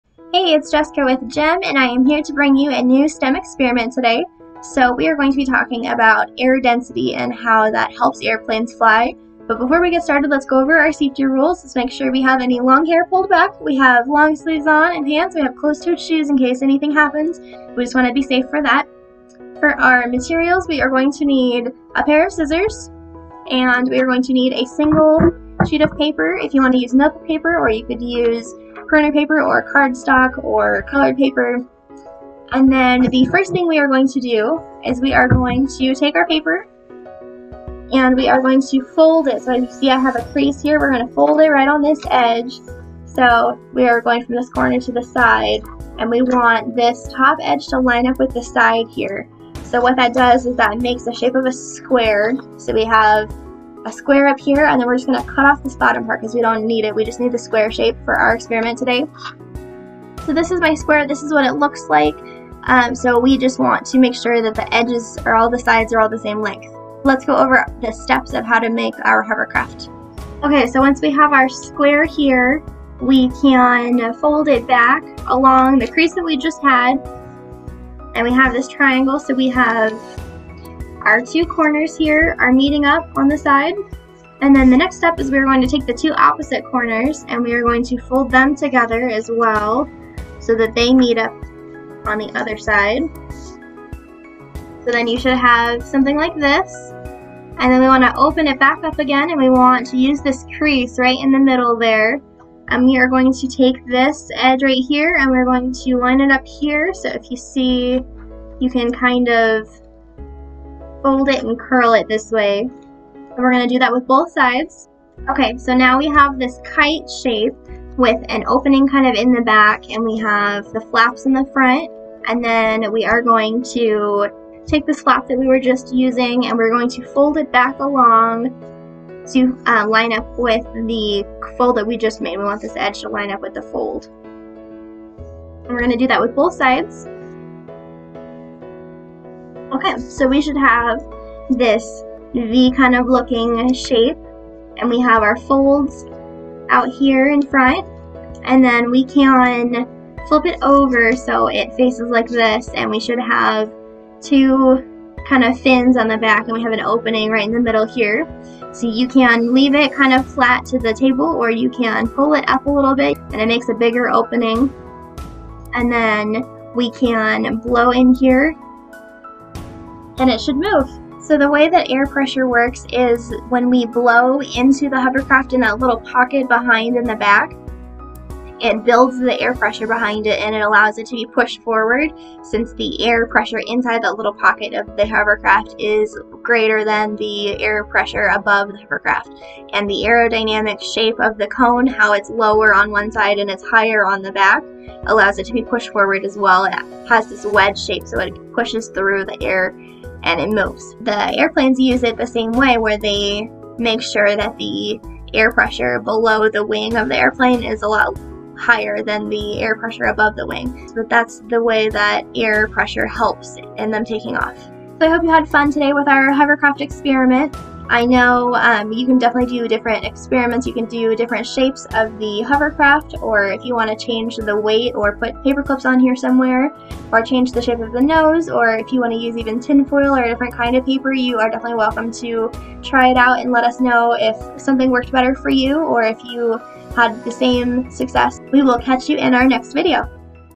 Paper Hovercraft
Youtube Video Description:
GEM Environmental is excited to share another science experiment! Join our AmeriCorps State Member Jessica and learn about how to make a paper hovercraft.
Share pictures, comments and questions on our Facebook page @GEMEnvironmental.